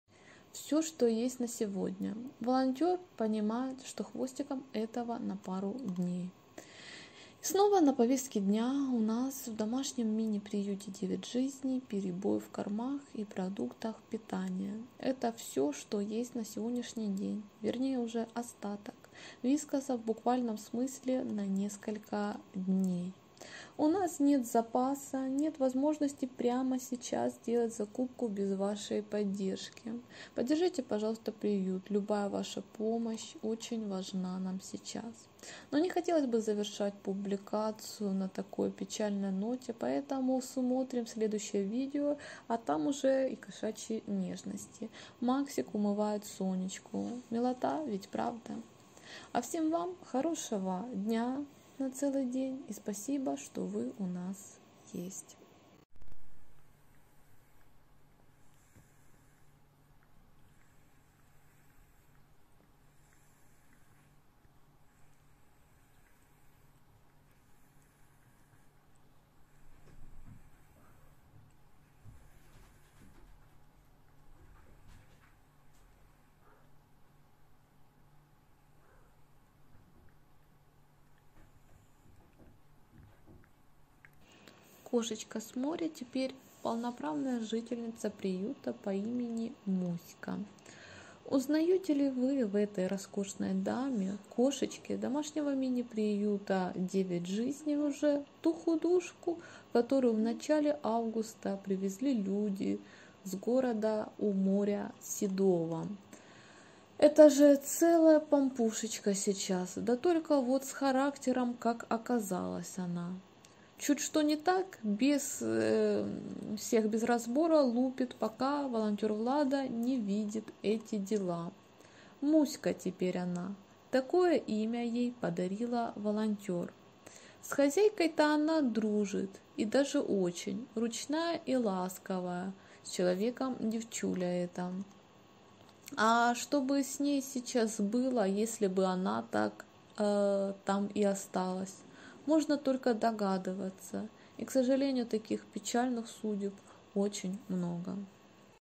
Запас на сегодня и как сейчас живет в приюте кошечка с моря Муська
Домашний мини-приют "Девять жизней", волонтер Влада

"Все что есть на сегодня: волонтер понимает, что хвостам этого на пару дней"